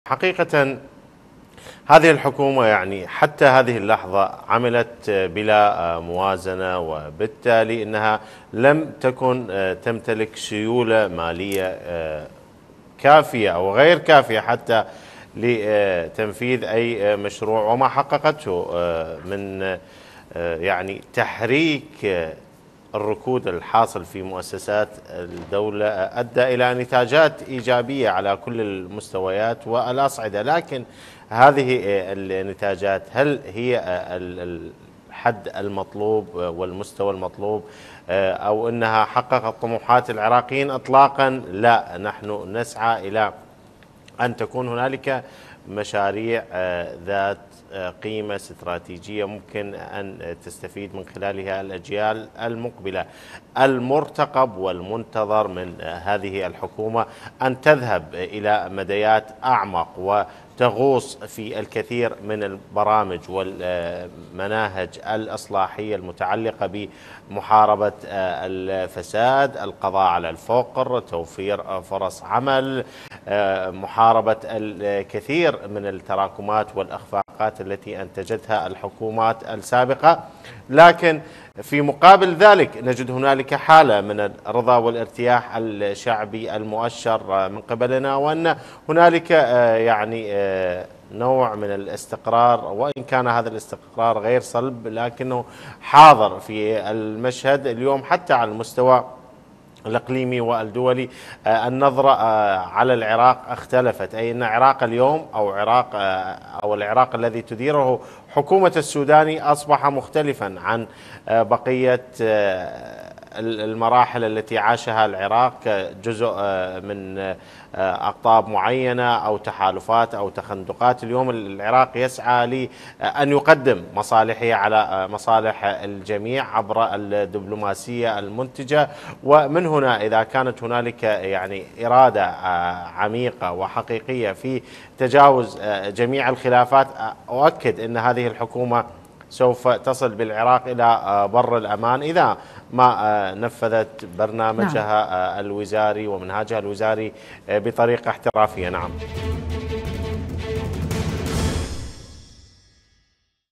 البيدر: السوداني حقق نتائج ايجابية رغم عدم امتلاكه السيولة المالية
موقع قناة التغيير الفضائية  الالكتروني